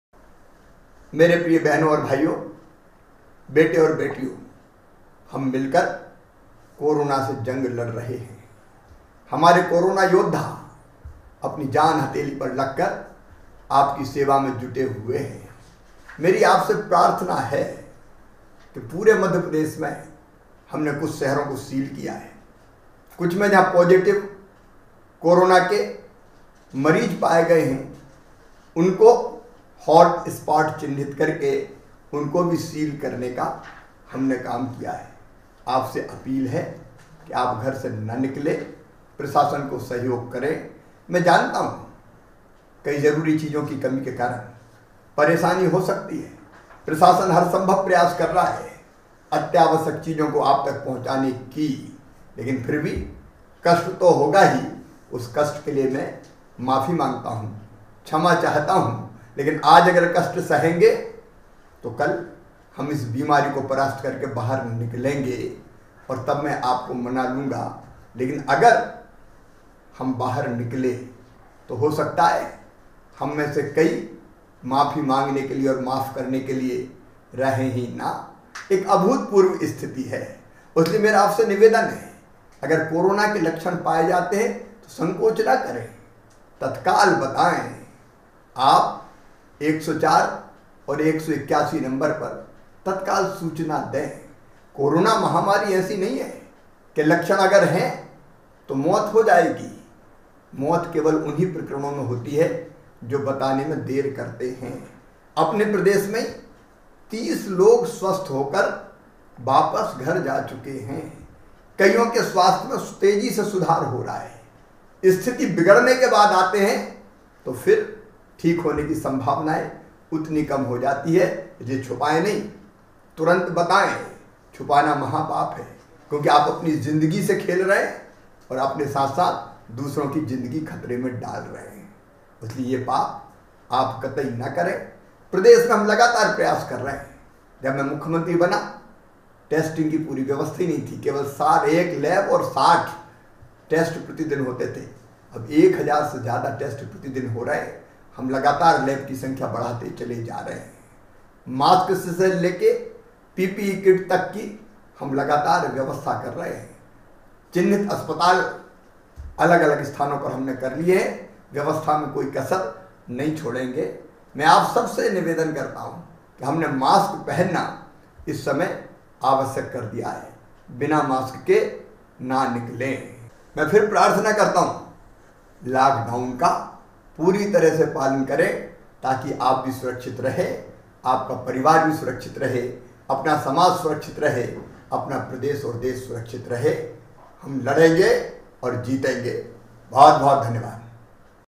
जो भी प्रतिबंध तोड़ेगा उस पर सख्त कार्रवाई होगी :मुख्यमंत्री श्री शिवराज सिंह चौहान
मुख्यमंत्री श्री शिवराज सिंह चौहान का कहना है कि घर से बाहर ना निकले अगर घर से बाहर निकले तो उस पर सख्त कार्रवाई की जाएगी और घर पर जाइए सुरक्षित रहिए अपने परिवार का ध्यान रखें